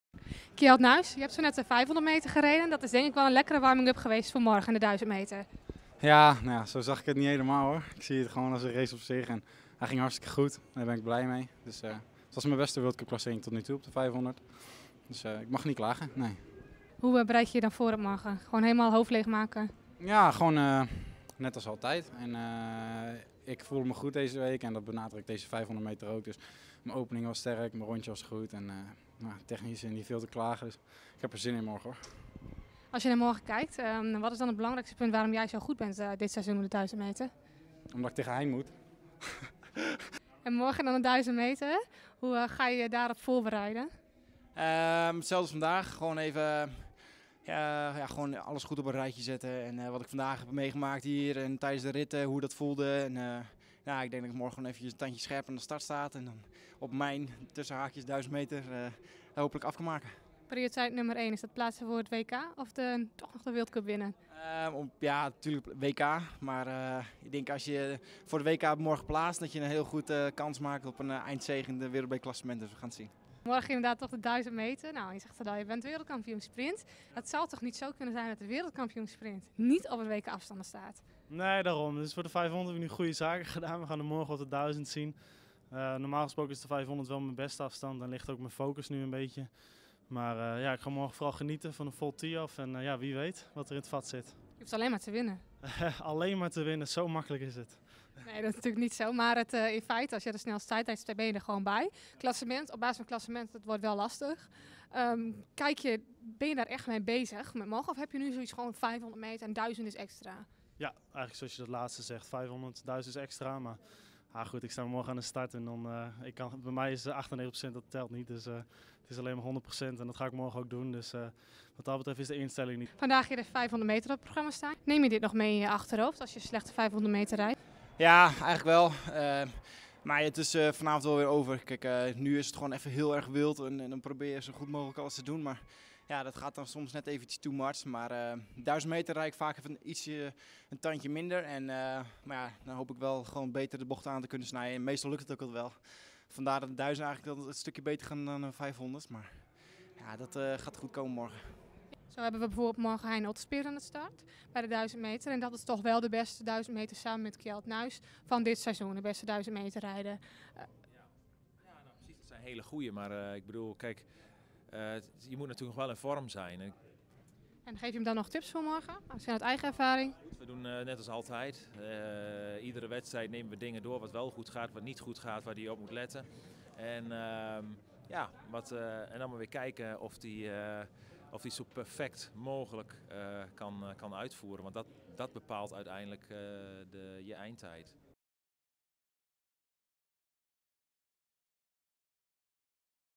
De voorbereiding op de 1000 meter
Op de 1000 meter staat er veel op het spel, een wereldbeker overwinning, het eindklassement en kwalificatie voor de Essent ISU WK afstanden in Sotsji. Hoe bereiden de mannen zich erop voor? Schaatsen.nl vraagt het aan Hein Otterspeer, Michel Mulder Kjeld Nuis en Gerard van Velde.